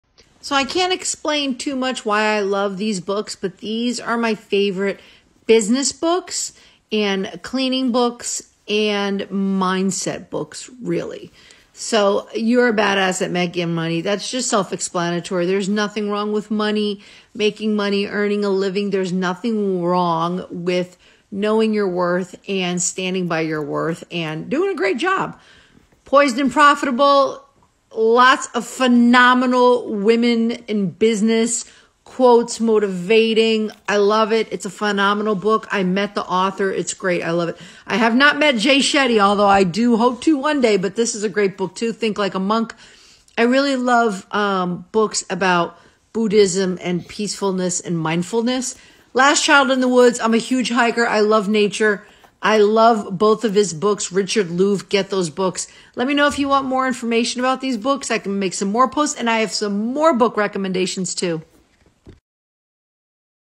My favorite business, entrepreneur, nature, motivating, money making books
I did end up explaining why I love the books, lol. Didn't think I'd have time. As a busy mom, wife and businesswoman, I need all the motivation I can get! I love reading uplifting and positive stories of successful entrepreneurs while also working on my spiritual side. What books do you recommend?

Here are my links to grab the books, great reads!!

Last Child
Vitamin N

Some of my favorite cleaning & organizing tool!